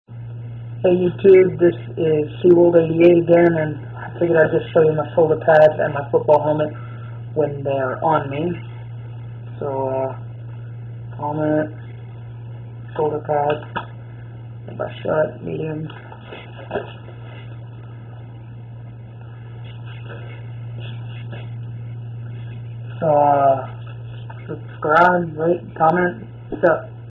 Me In My Helmet And Shoulder Pads
Sorry for the bad quality, vid was uploaded from my phone.
Helmet is a Riddell Revolution w/ Lineman Facemask
Pads are Schutt Varsity Flex